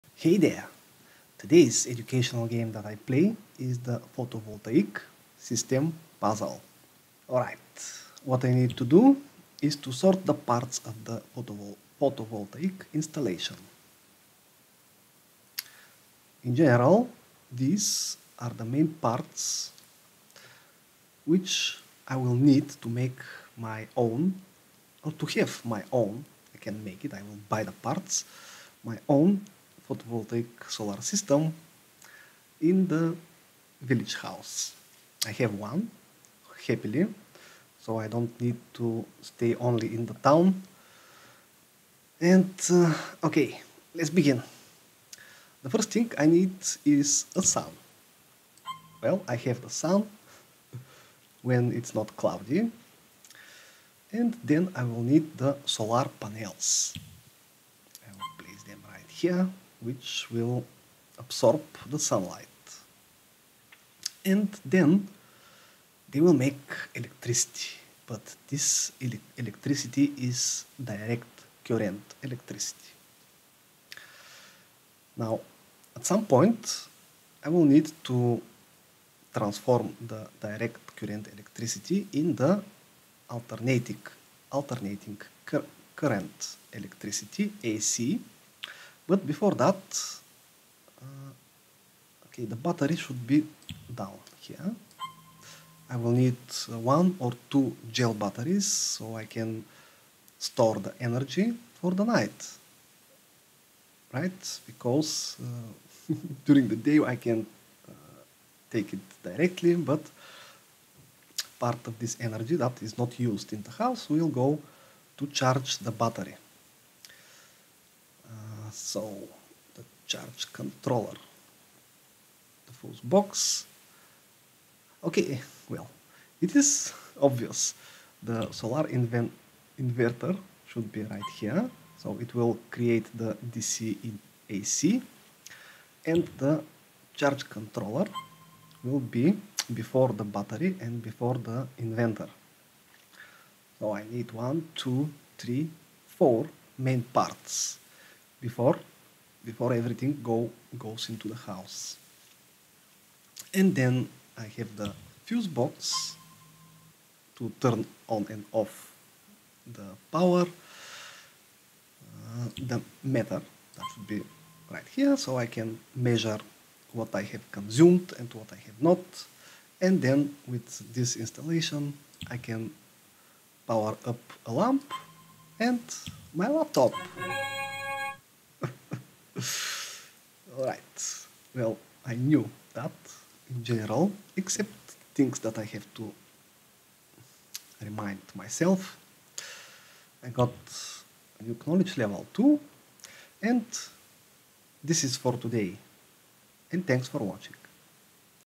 Structure of Photovoltaic System - Interactive Physics Lesson 🚀🎥🎓
Fun educational video about the parts of the photovoltaic installation. Suitable for online lessons, interactive classes and exciting homework.